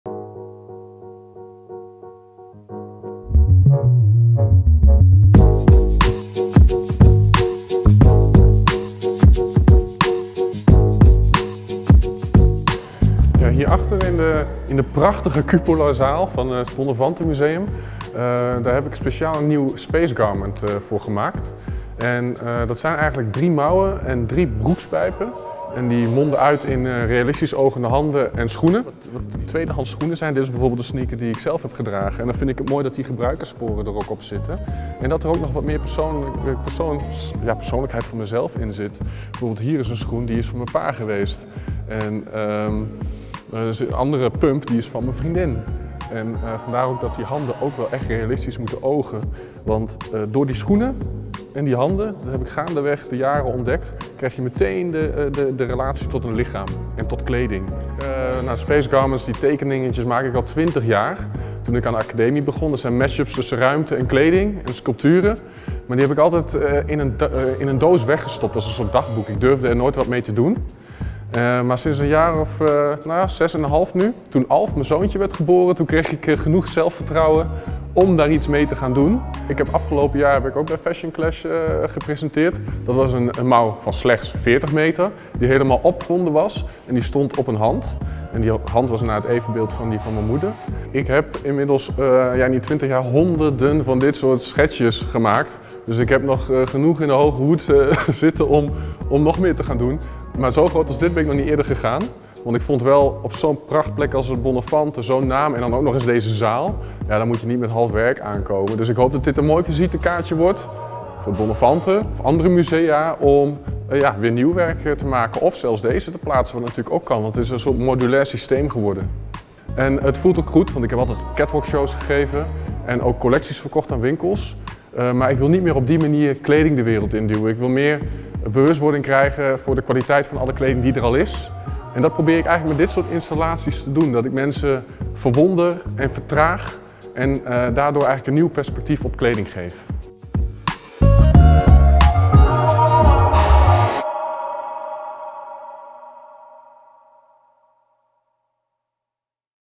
A landscape of 280 meter sleeves and jeans at Bonnefantenmuseum.
Space Garment by Antoine Peters
Commissioned by FASHIONCLASH Maastricht
Hosted by Bonnefantenmuseum Maastricht
Video by Daniel van Hauten
Made possible by Creative Industries Fund NL

A little more context;
I created collections for the catwalk for a long time. Today, my objects move between fashion, art and architecture, in which the relationship between clothing and the human body is a binding factor. For 20 years, I have been drawing hundreds of site-responsive garments and sculptures. I have always kept the sketches hidden like a diary. But since the birth of our son Alf, I dare to make these Space Garments a reality.

The space around a garment is just as important as the garment itself!

Clothing – the ultimate universal visual language – is distorted, stretched, repeated, cropped and reconfigured in all sorts of ways and surprises the viewer with a new visual language. With my work, I want us to look differently at clothing, the world around us and ourselves, literally and figuratively. In doing so, I break through the traditional relationship between the viewer and the object.

Clothing is so fascinating because it’s close to the body, and has the power to influence your movement, feeling or surrounding, but at the same time it doesn’t have to be functional at all. A garment does not need to obey the body or even needs a physical host to obtain its power or even be móre powerful!

My curious objects have a disruptive effect on the viewer. They ask for delay and a change of perspective. In this way I offer a counterbalance to consumerism and ensure an emotional reappraisal of the many items of clothing that are already there, while simultaneously questioning personal space and intimacy.